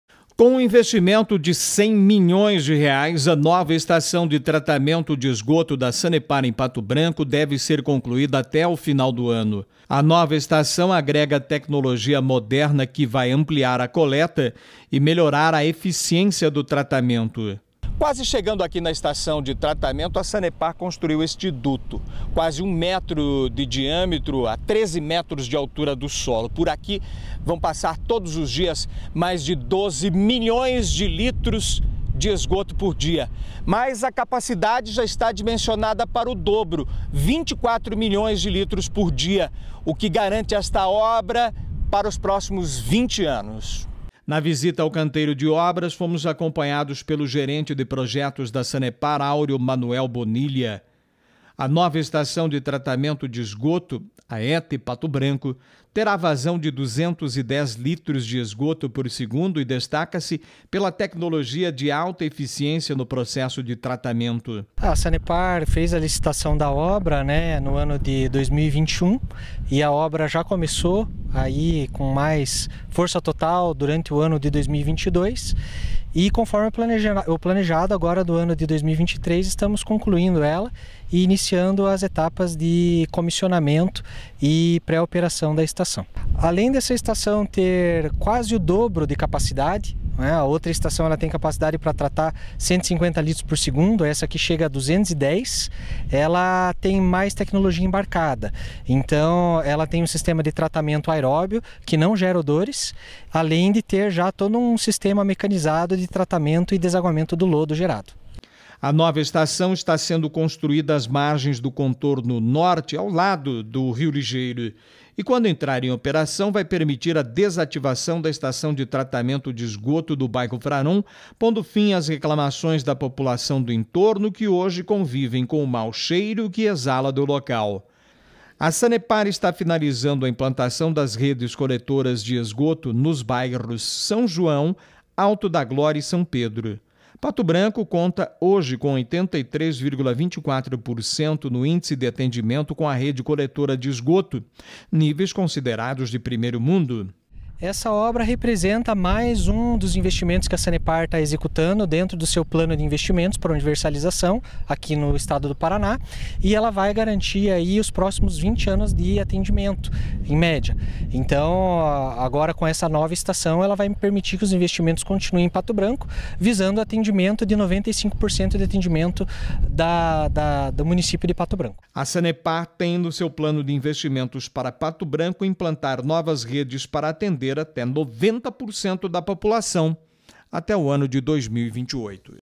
Sanepar deve concluir nova estação de tratamento de Pato Branco até o final deste ano
Gestão Pública: Nova estação de tratamento de esgoto de Pato Branco deve ser concluída até o final deste ano.